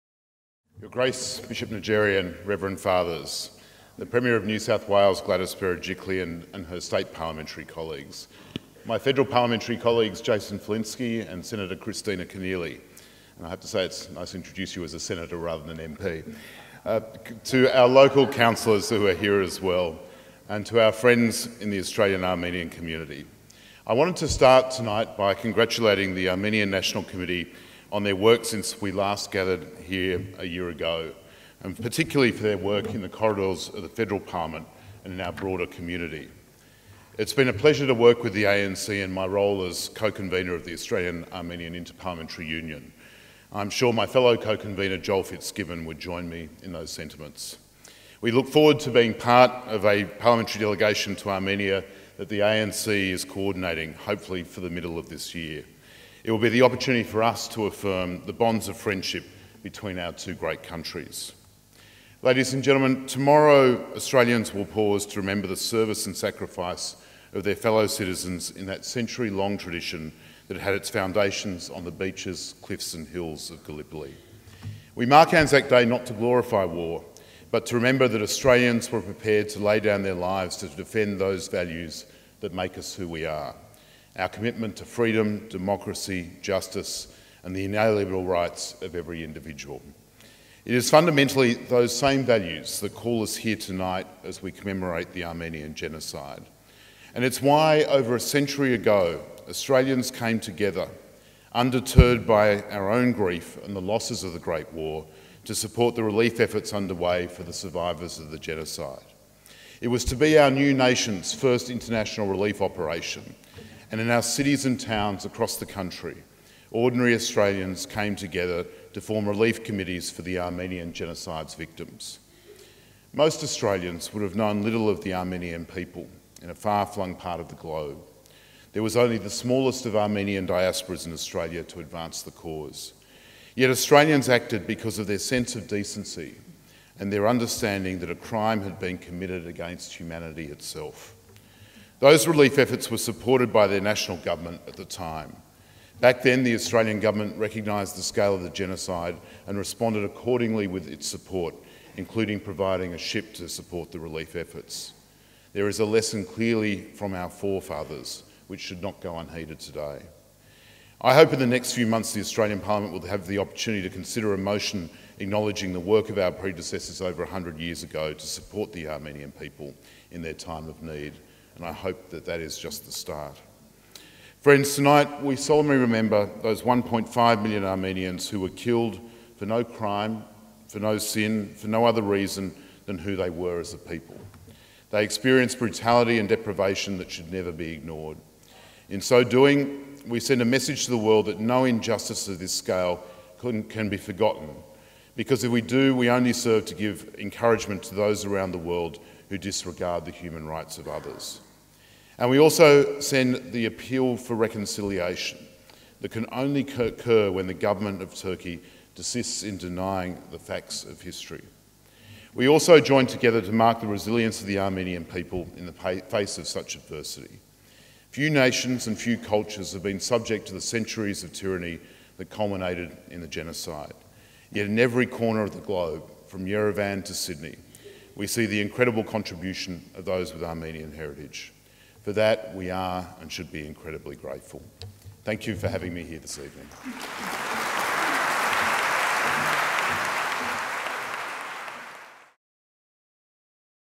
Federal MP Trent Zimmerman - 2018 Sydney Armenian Genocide Commemoration
Federal Parliamentarian and Chair of the Federal Australia-Armenia Inter-Parliamentary Union, Trent Zimmerman MP addresses the audience at the 2018 National Armenian Genocide Commemoration at The Concourse in Sydney.